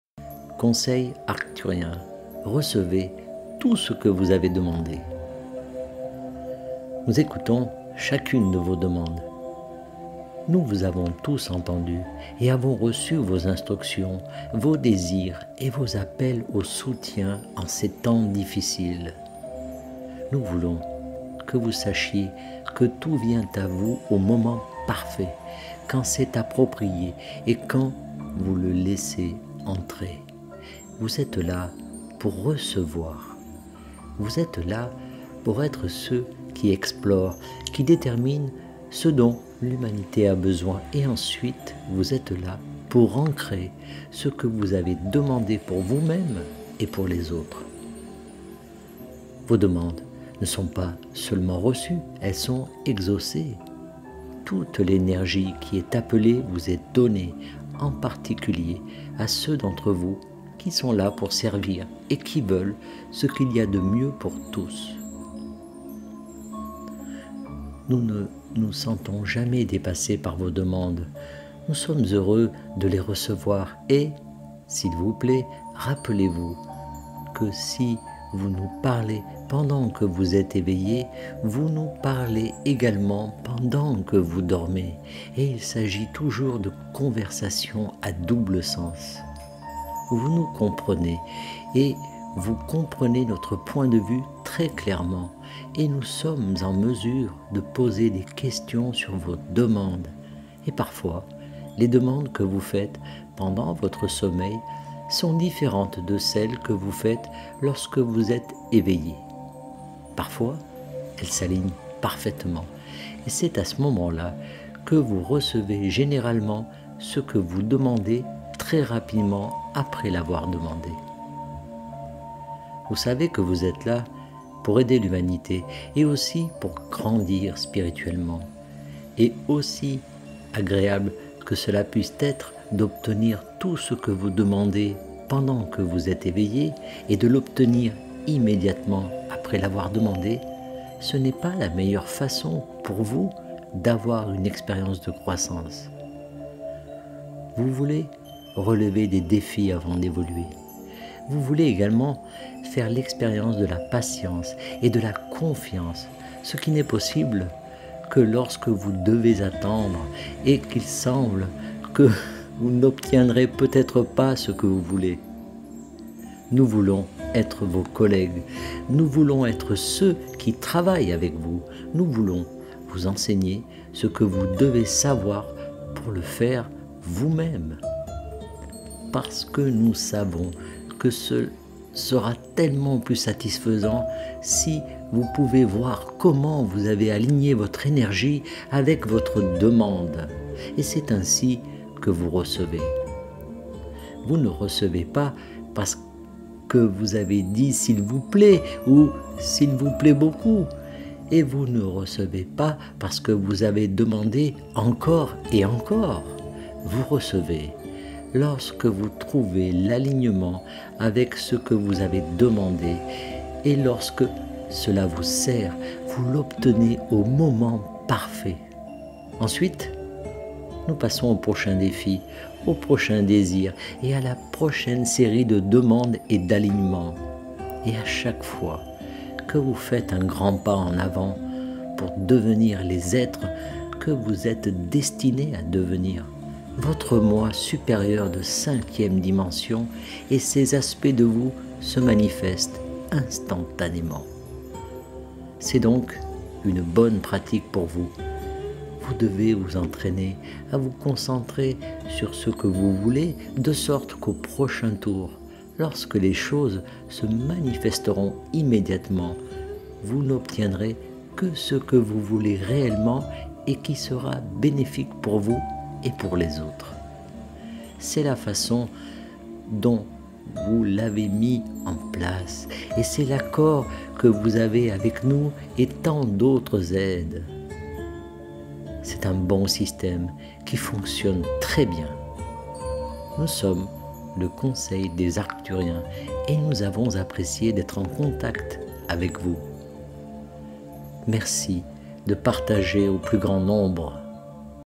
Conseil Arcturien : Recevez tout ce que vous avez demandé
Nous écoutons chacune de vos demandes. Nous vous avons tous entendus et avons reçu vos instructions, vos désirs et vos appels au soutien en ces temps difficiles. Nous voulons que vous sachiez que tout vient à vous au moment parfait, quand c’est approprié et quand vous le laissez entrer. Vous êtes là pour recevoir.